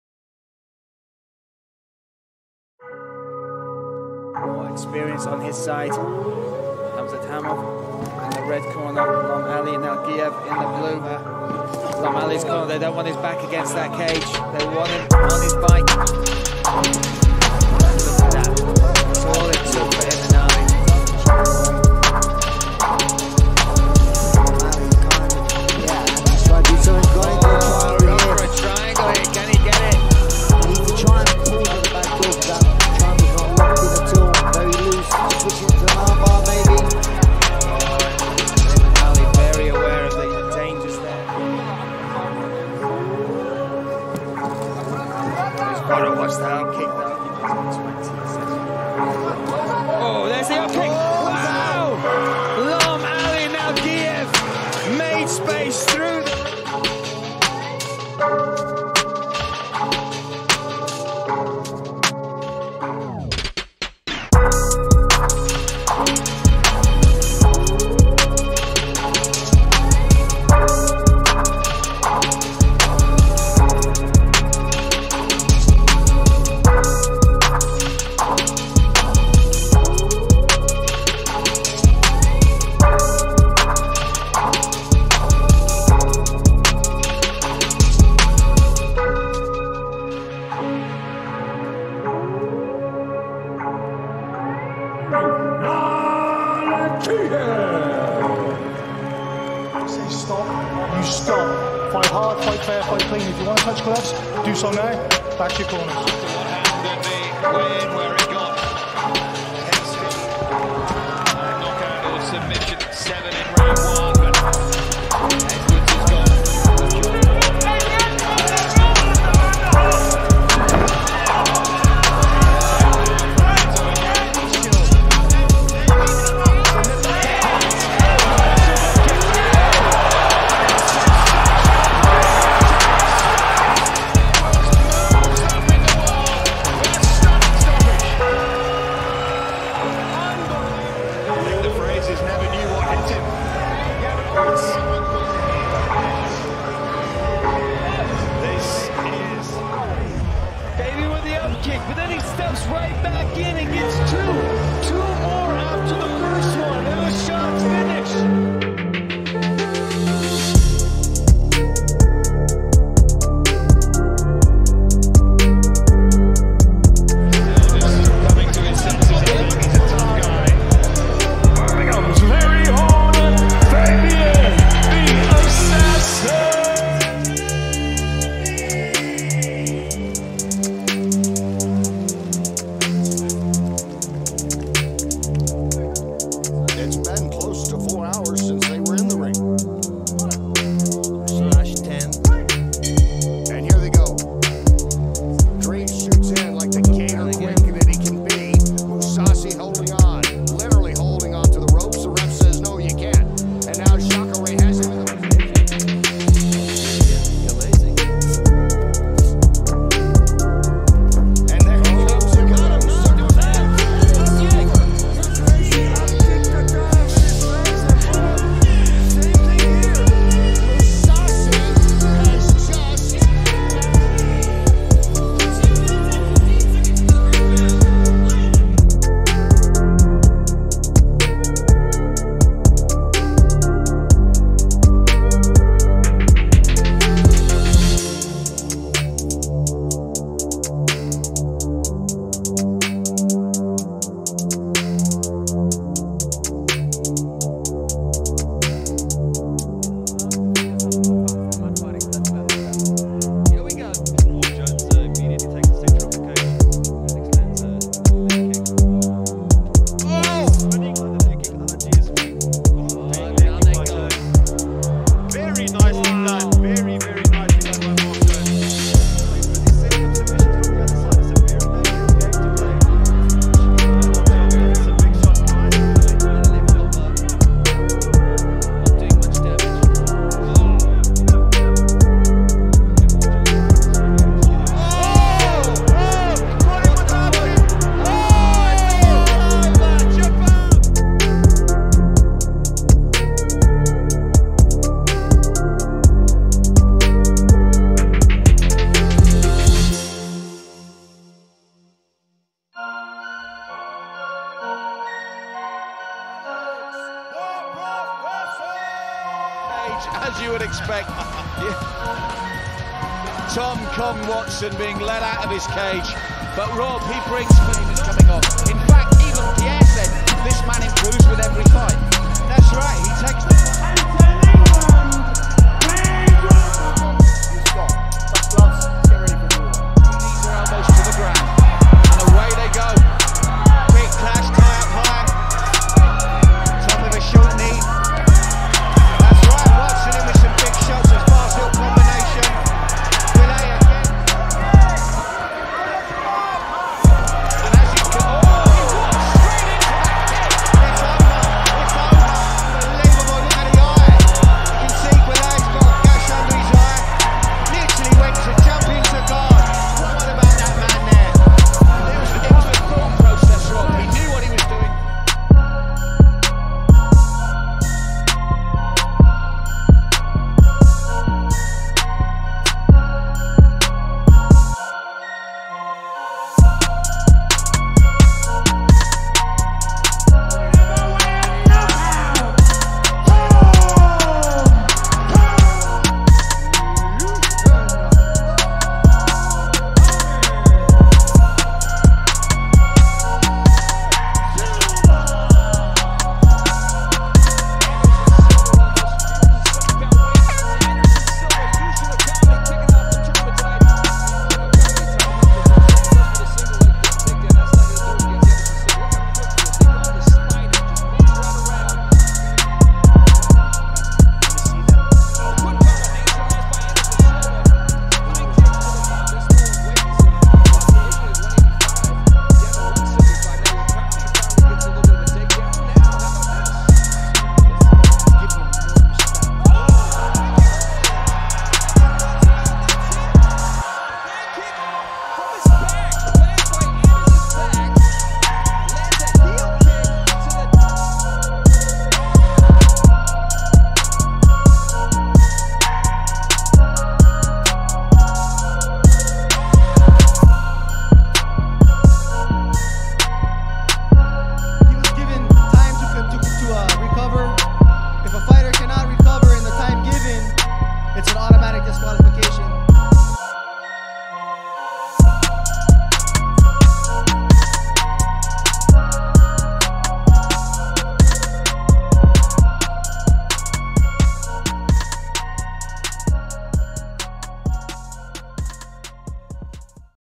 BEST UPKICKS EVER - KNOCKOUTS COMPILATION ▶ HIGHLIGHTS MMA [HD] vol. 1
BEST UPKICKS EVER - KNOCKOUTS COMPILATION ▶ HIGHLIGHTS MMA [HD]
UPKICK FINISHES, KNOCKOUTS, BRUTAL AND INSANE KICKS
== SEE ALSO ==
▶ KHABIB'S VERY EMOTIONAL REACTION TO THE DEFEAT OF HIS BEST FRIEND ▶ Shamil Zavurov vs Nariman [HD]